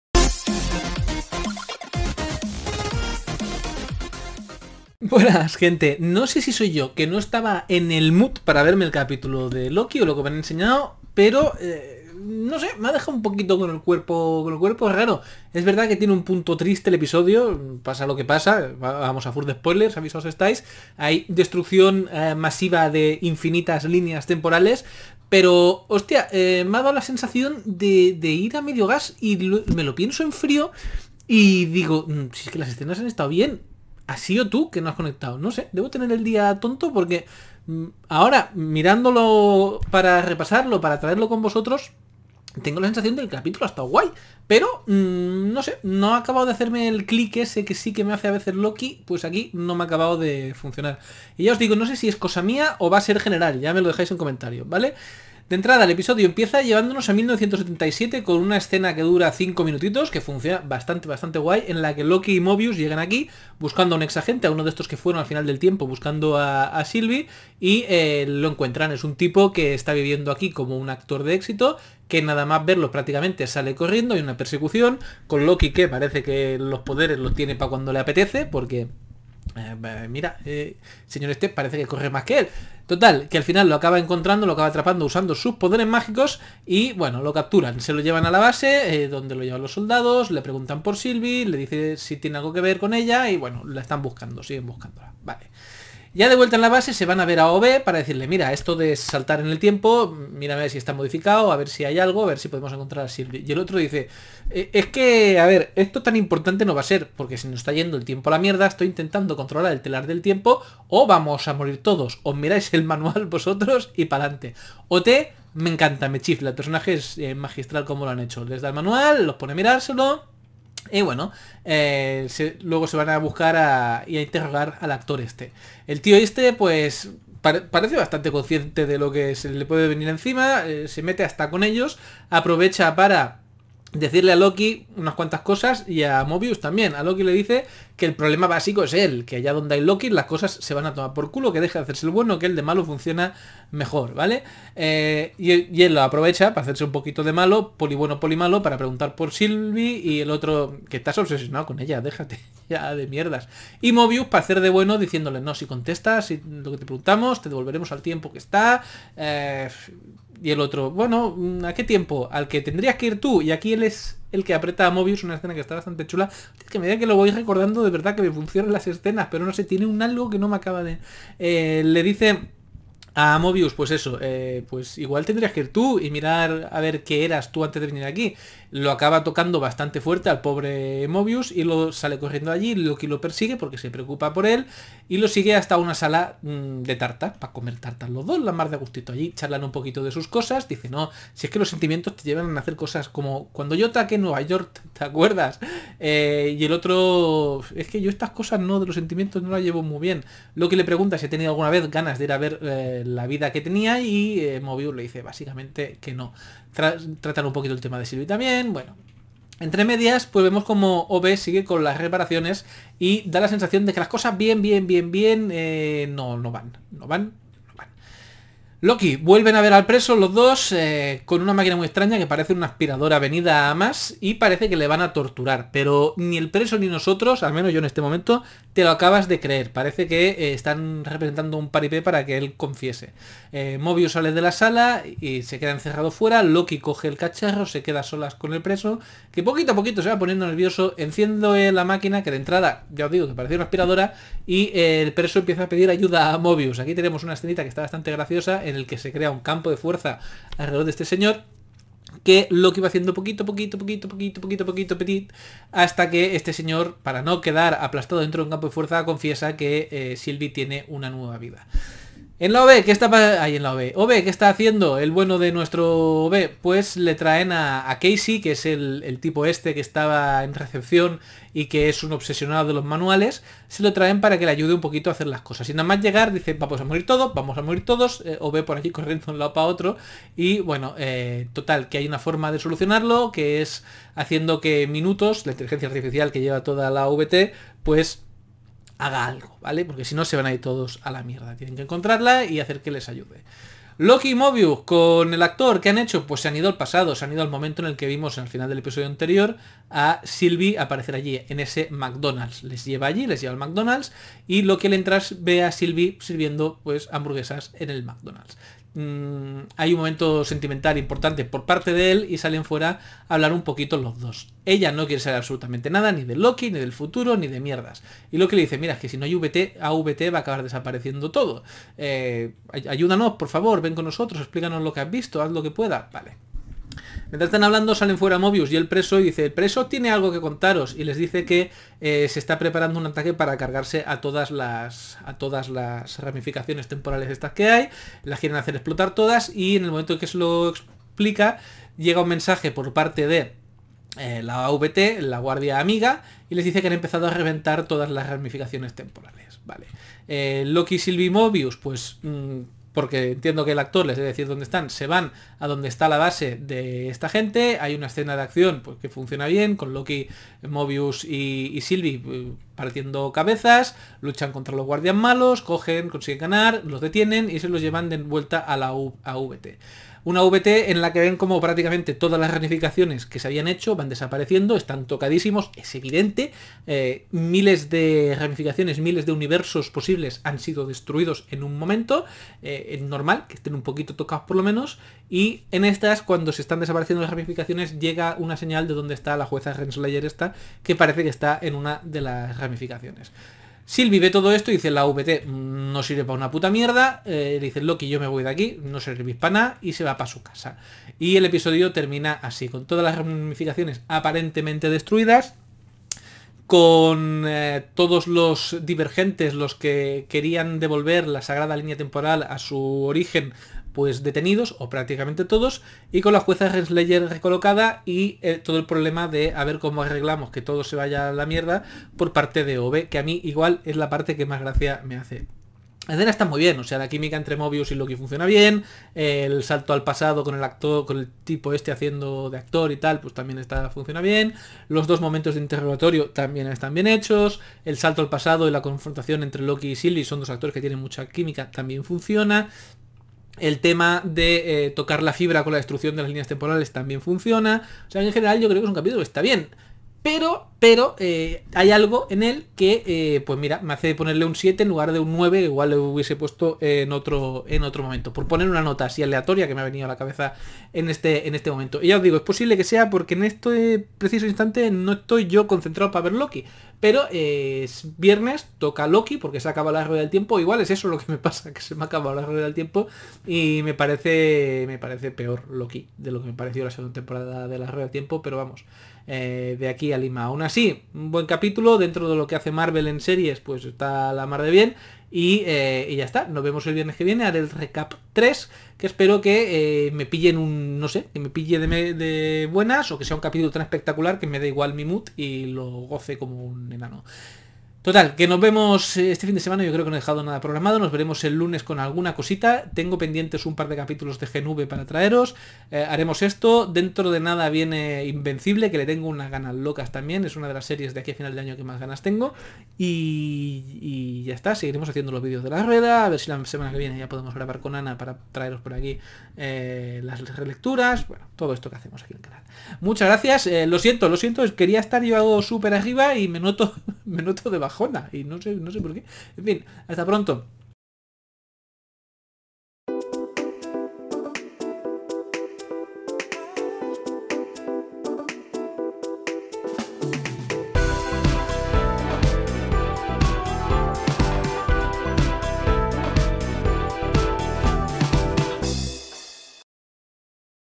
Loki: Temporada 2, Episodio 2. Poli bueno, poli malo
Sigue Loki adelante con un capítulo bastante entretenido. Os ha gustado?